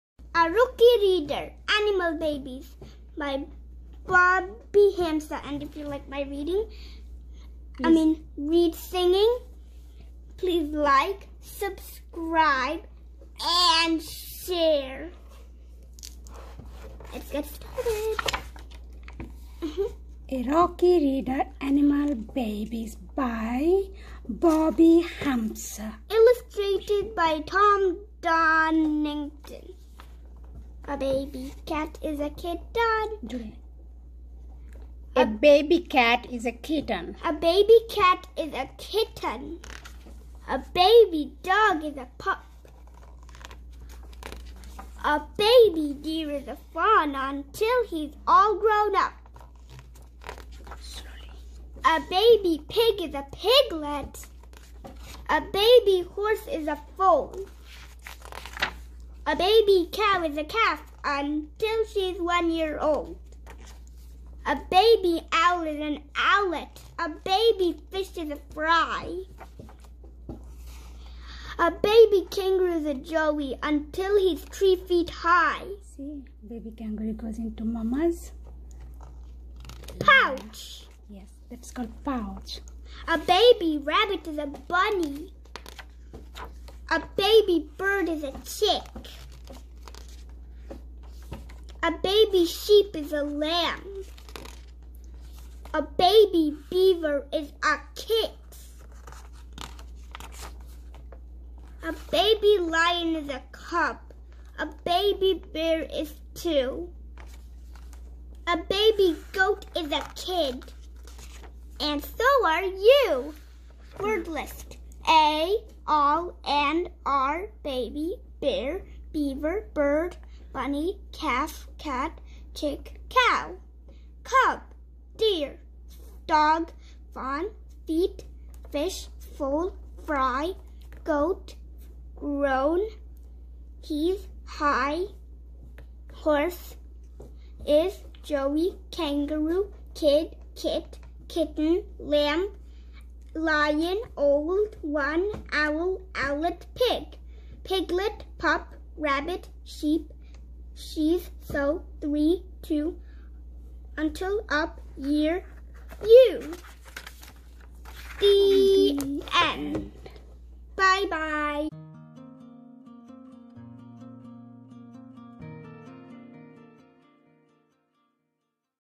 Read with me: ANIMAL BABIES | By Bobbie Hamsa | Early Readers | SistersLoves
👏👏👏 Read with my six years old daughter! This is the very first step to make your children love to read, as more as you can read with them. It's not only help your children reading, also will improve speaking and thinking ability. High - interest stories make reading fun! Stories have been tested for vocabulary and sentence length to help you make the right choice! A letter from an education specialist gives you valuable advice on how to read to your child and how to listen to your child read to you!

🏆🏆🏆"You are your child's first teacher ... the most important one!", said Priscilla Lynch, Ph.D.

About this book:
ANIMAL BABIES
By Bobbie Hamsa
A Rookie Reader TM
Level: Pre-reader Level B

A baby kangaroo is a joey. A baby fish is a fry. But no matter what they are called, all baby animals are adorable as they jump, scamper, and swim in their habitats. Learn the different baby animals' names so you can share which one is your favorite.